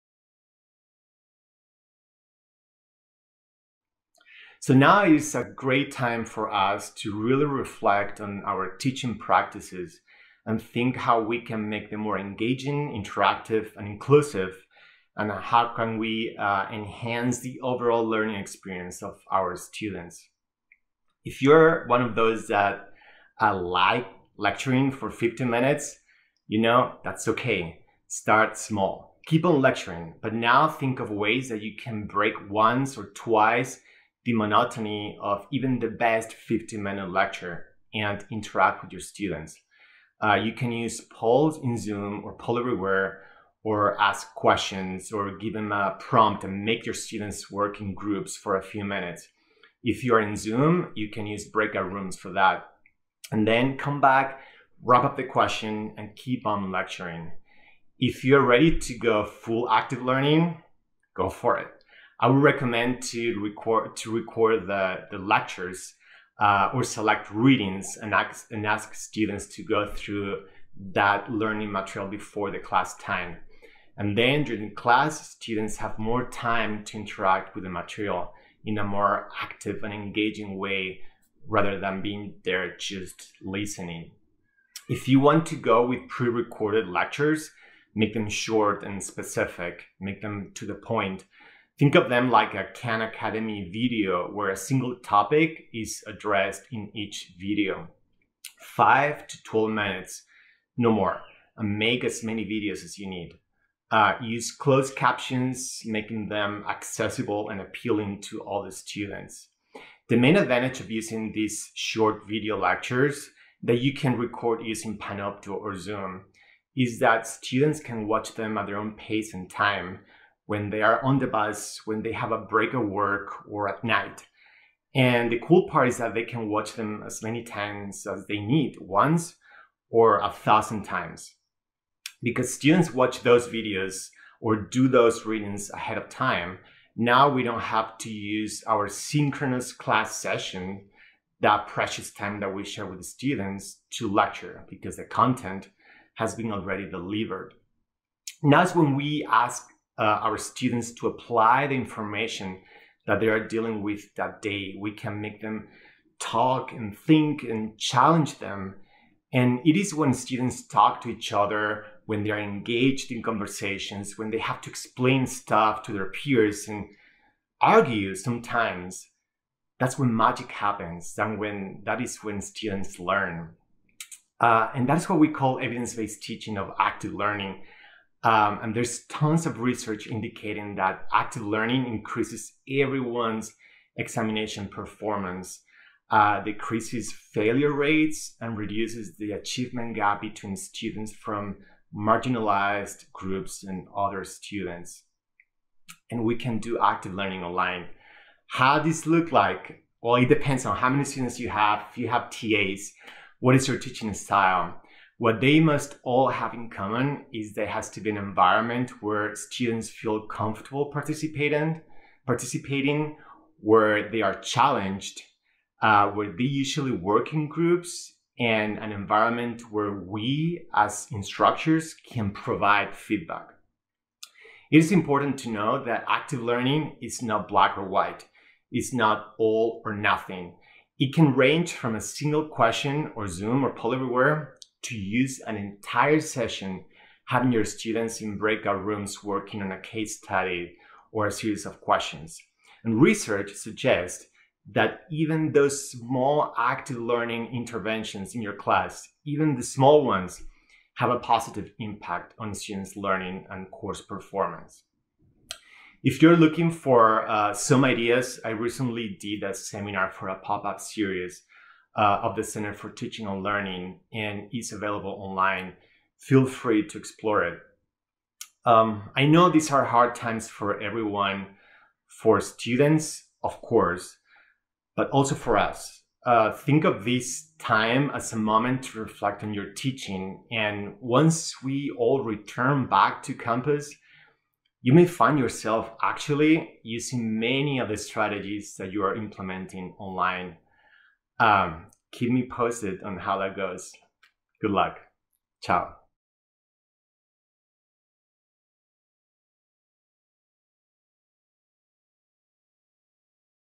Evidence Based Teaching Practices José M Guzmán
Explore evidence-based teaching practices for making classes more engaging, interactive and inclusive with Assistant Teaching Professor José M Guzmán of the University of Washington’s School of Aquatic and Fishery Sciences.